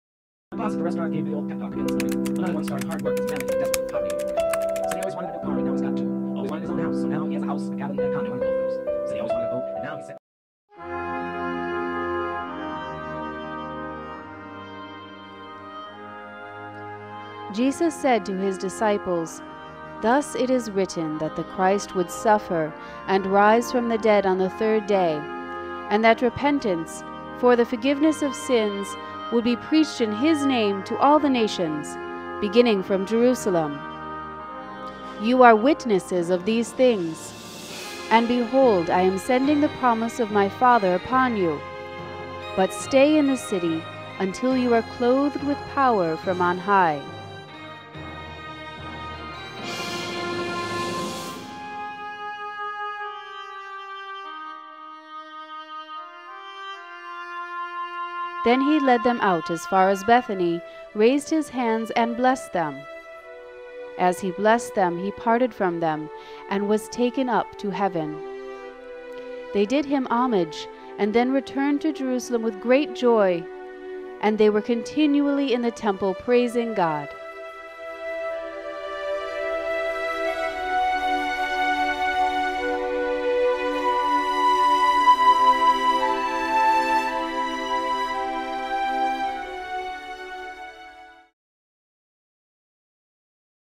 Gospel for Sunday, May 16th - The Ascension of the Lord
Lk 24:46-53Jesus said to his disciples: "Thus it is written that the Christ would suffer and rise from the dead on the third day and that repentance, for the forgiveness of sins, would be preached in his name to all the nations, beginning from Jerusalem. You are witnesses of these things. And behold I am sending the promise of my Father upon you; but stay in the city until you are clothed with power from on high."Then he led them out as far as Bethany, raised his hands, and blessed them. As he blessed them he parted from them and was taken up to heaven. They did him homage and then returned to Jerusalem with great joy, and they were continually in the temple praising God.